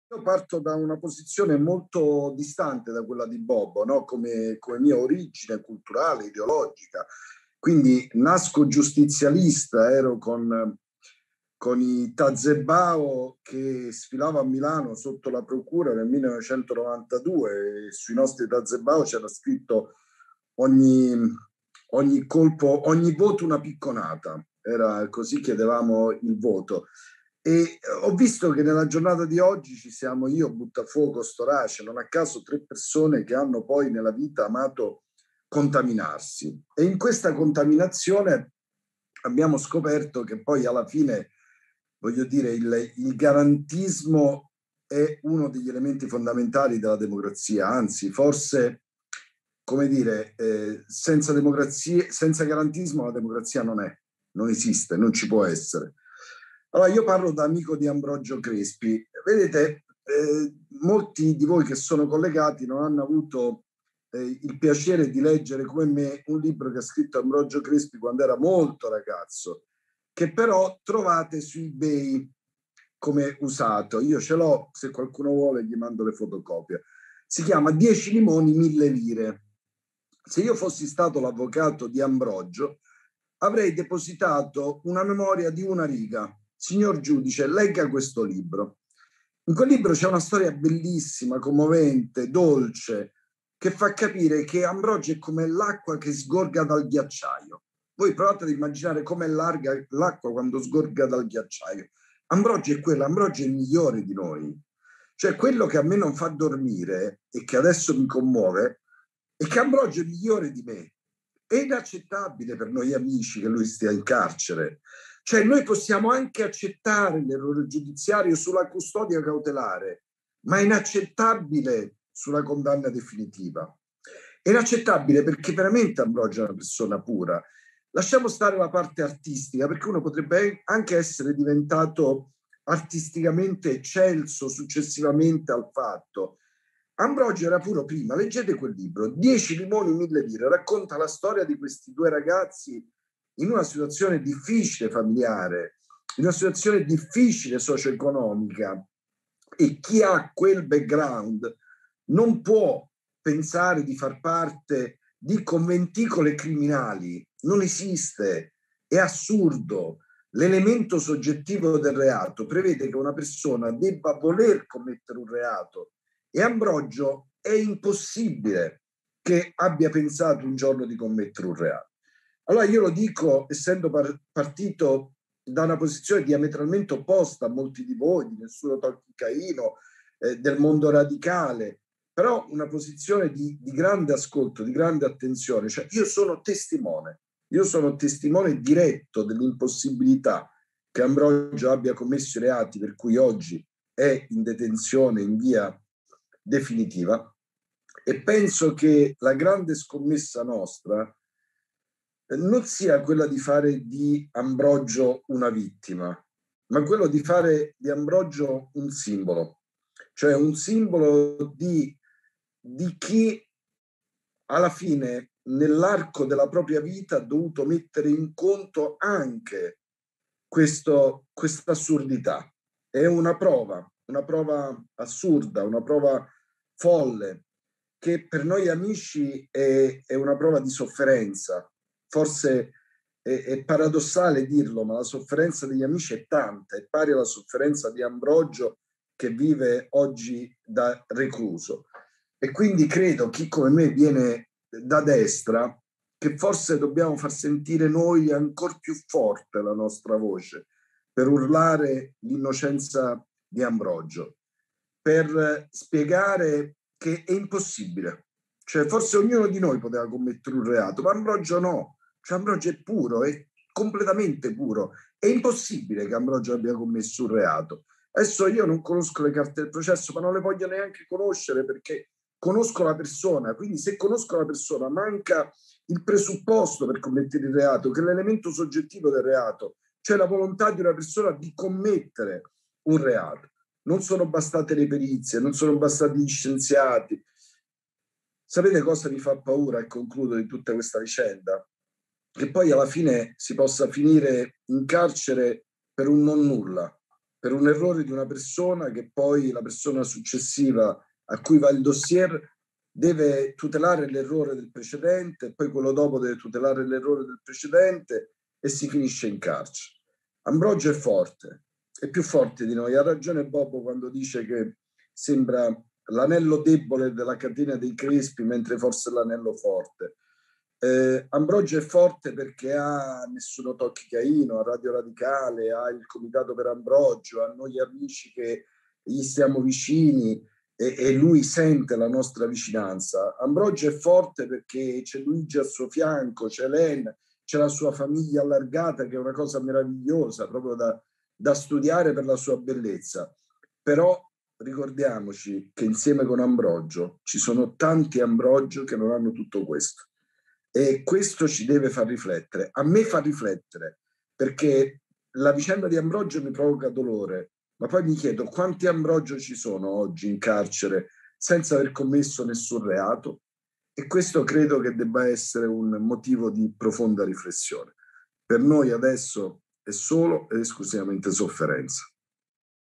Italo Bocchino - Maratona oratoria per Ambrogio Crespi
L'intervento di Italo Bocchino, profondo ed incisivo, per sostenere l'innocenza di Ambrogio Crespi durante la maratona oratoria su Radio Radicale organizzata dal Comitato Ambrogio Crespi di Nessuno tocchi Caino.